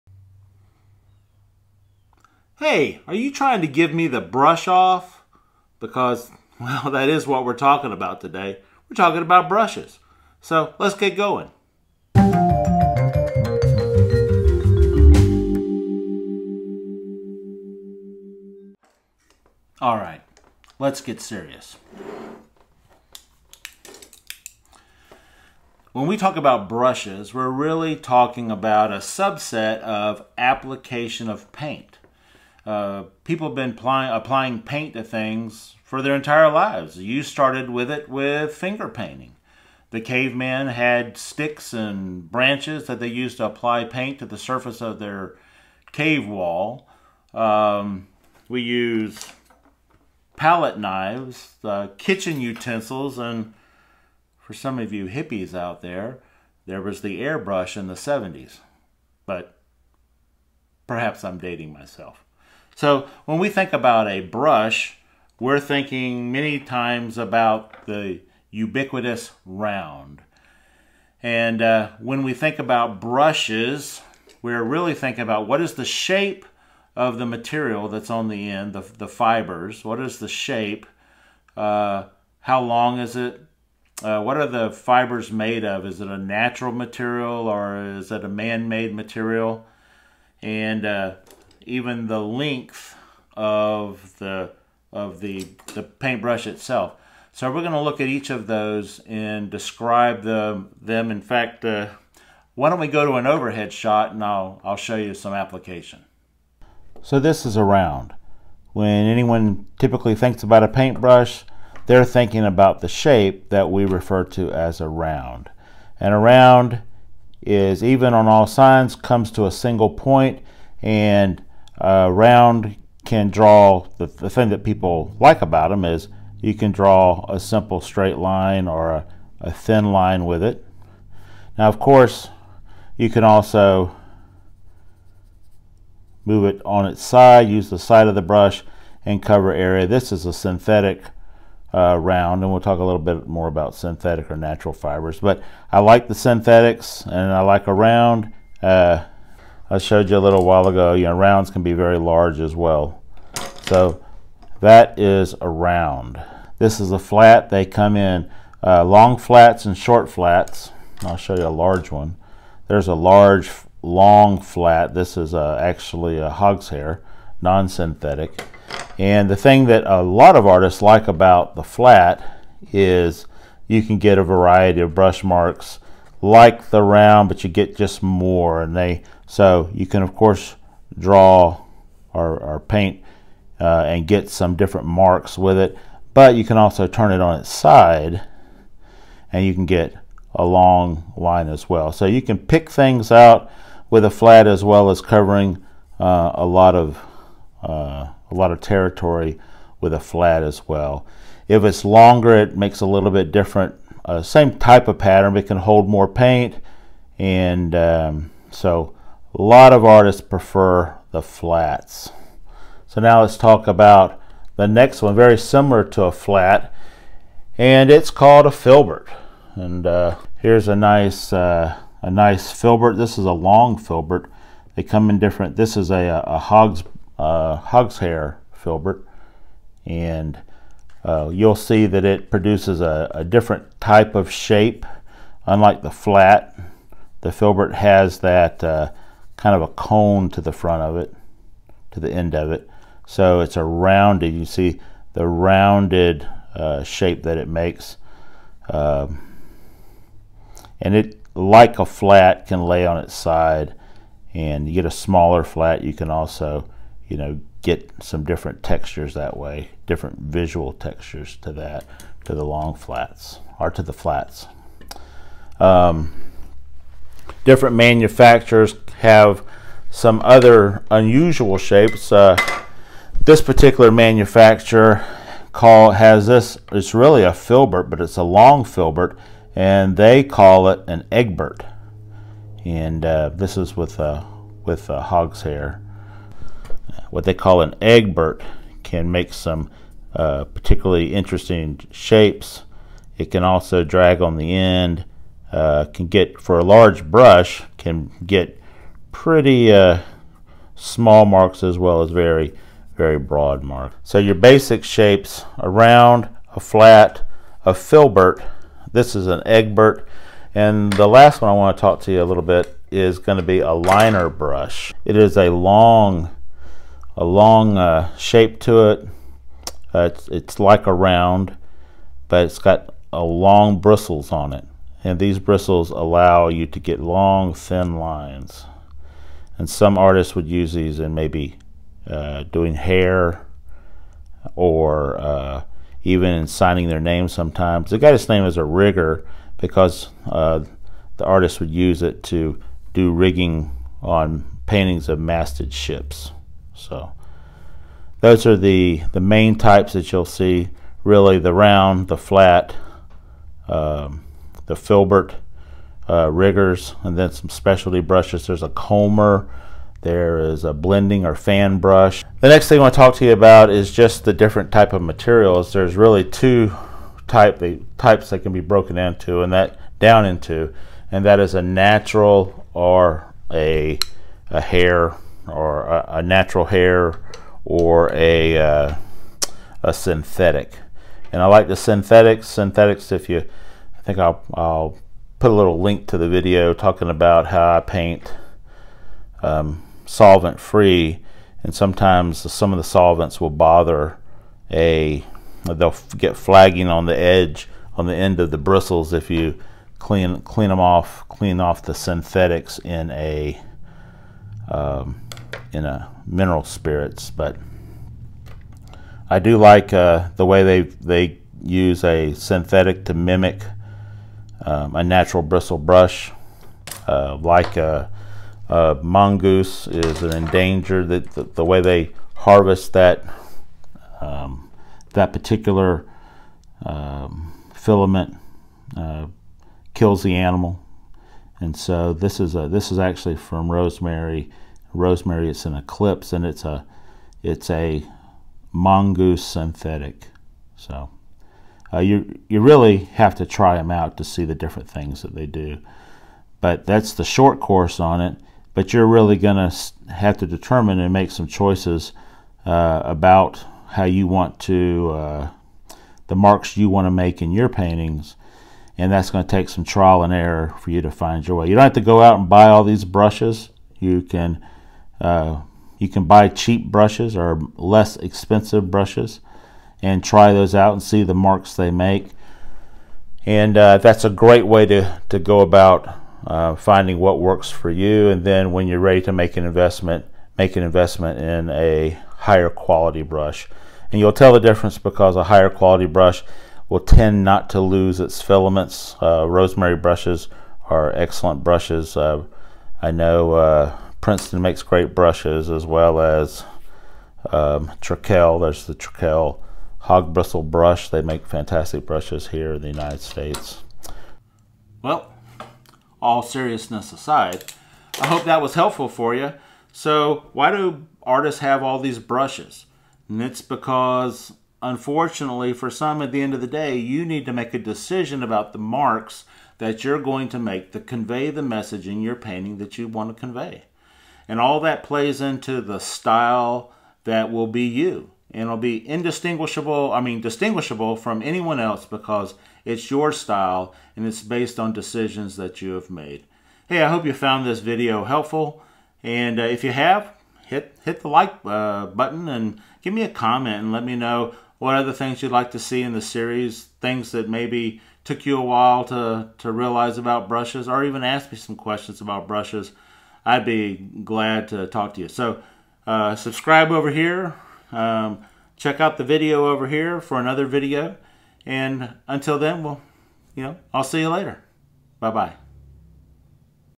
Choose Your Best Brush for Painting... A guide for Beginners
A short tutorial to introduce the basic paint brush profiles.  You'll get to see the marks the individual brushes make and perhaps help you choose the brush that'll define your style.

Got questions??? Put it in the comments.  I'd love to hear from you.